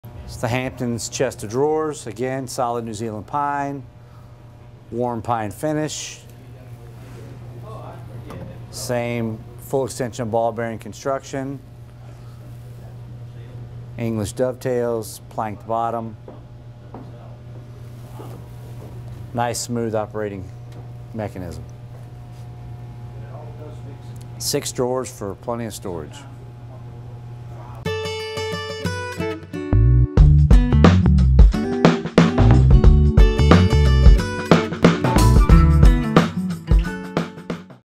Hillsdale Hamptons 5-Drawer Dresser
• Solid New Zealand pine
• Full extension drawers
• Planked bottom drawers
• Offers plenty of storage